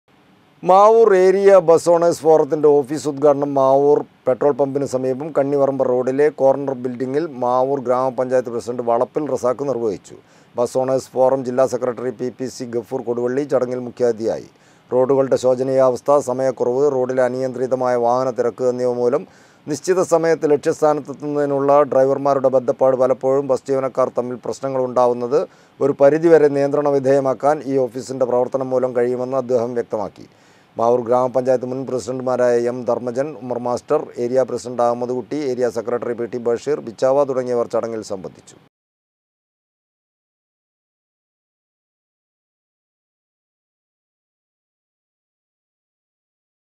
മാവൂർ ഏരിയ ബസ്സ് ഓണേഴ്സ് ഫോറത്തിൻ്റെ ഓഫീസ് ഉത്ഘാടനം മാവൂർ പെട്രോൾ പമ്പിന് സമീപം
മാവൂർ ഏരിയ ബസ്സ് ഓണേഴ്സ് ഫോറത്തിൻ്റെ ഓഫീസ് ഉത്ഘാടനം മാവൂർ പെട്രോൾ പമ്പിന് സമീപം കണ്ണിപറമ്പ റോഡിലെ കോർണർ ബിൽഡിങ്ങിൽ മാവൂർ ഗ്രാമപഞ്ചായത്ത് പ്രസിഡണ്ട് വളപ്പിൽ റസാഖ് നിർവ്വഹിച്ചു